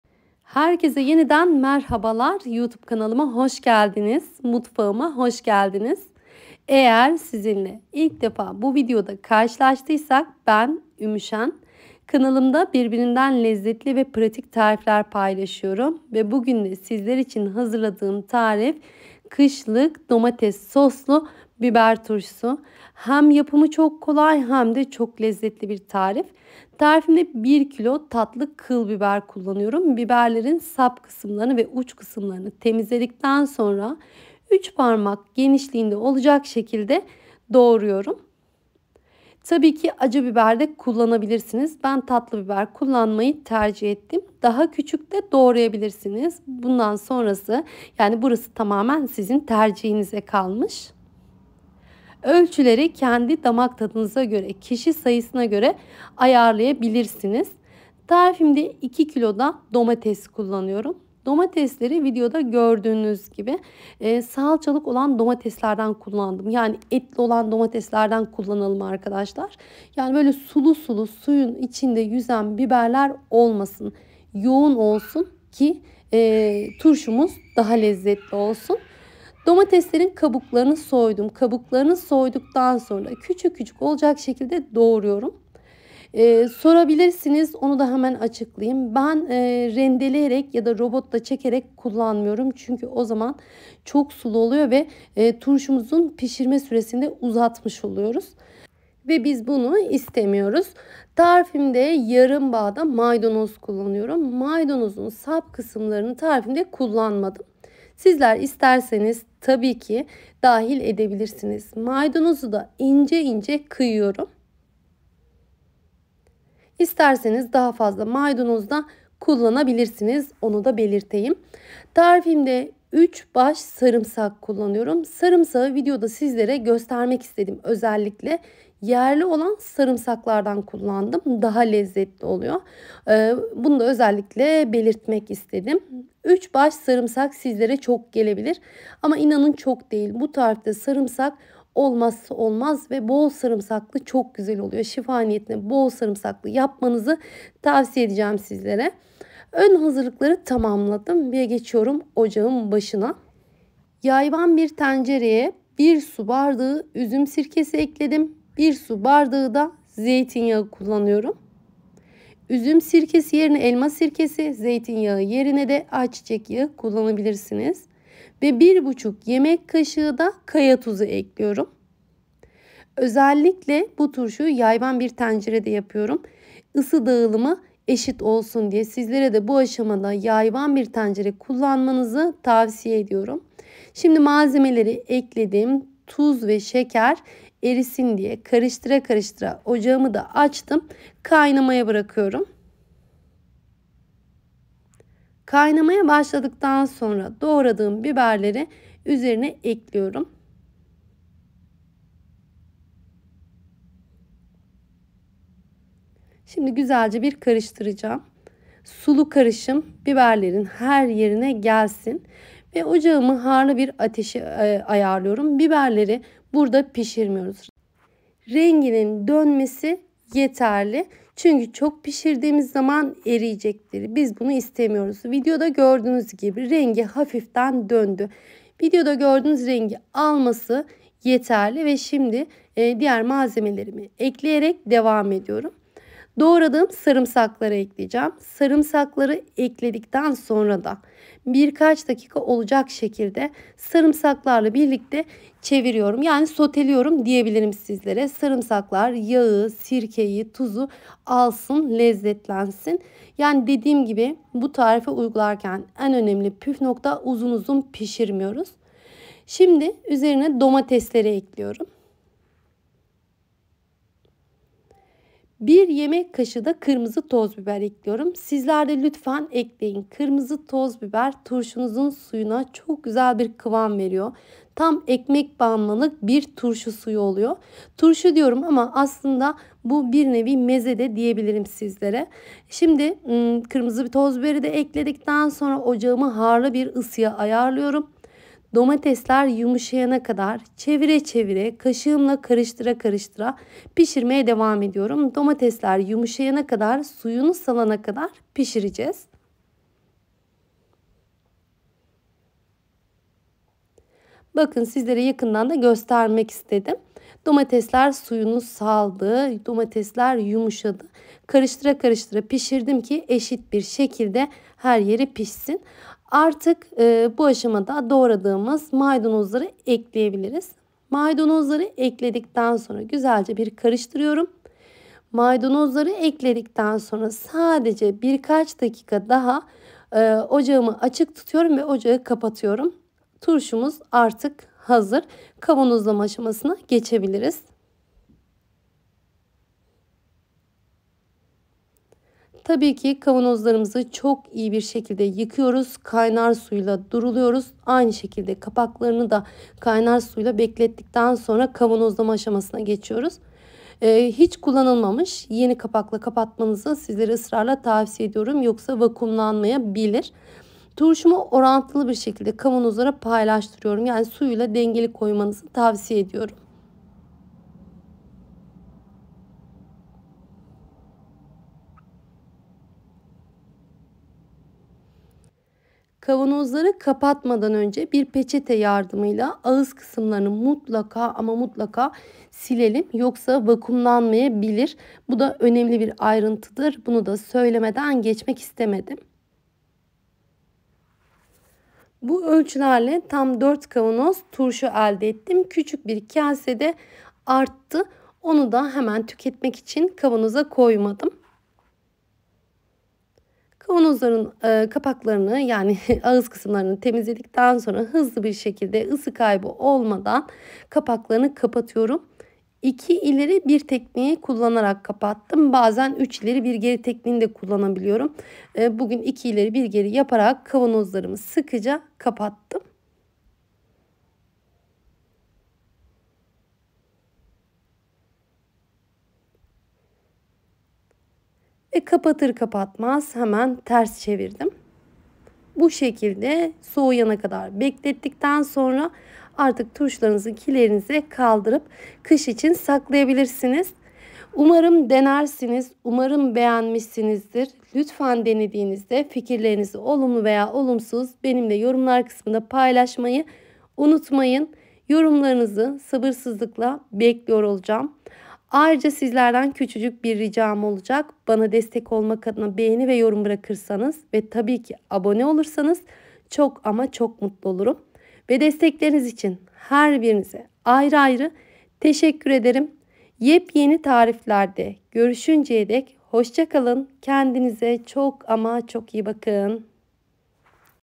4K | I will make a pickle | I am getting ready for winter | Please Enable Subtitles ⚙🌏
Subtitles: The video has subtitles in every language.
Please enable subtitles in your language from settings  ⚙🌏🌎🌍🗺.
4K Ultra hd Video. 4K 60 fps Video. 4K UHD 60 fps
Pickle recipes. How is pickle made? Tomato pickle. Pepper pickle. Pickled peppers with tomato sauce.

I love💖everyone

A list of materials :
1 kg of green pepper
2 kg of industrial tomatoes
Half a bunch of parsley
35 cloves of garlic
1 water glass of vinegar. ( 200 ML ).
1 water glass of liquid vegetable oil. ( 200 ML ).
1,5 tablespoons of rock salt. ( 50 grams ).
1 tablespoon of sugar. ( 15 grams ).
1 tablespoon of red powdered pepper. ( 15 grams ).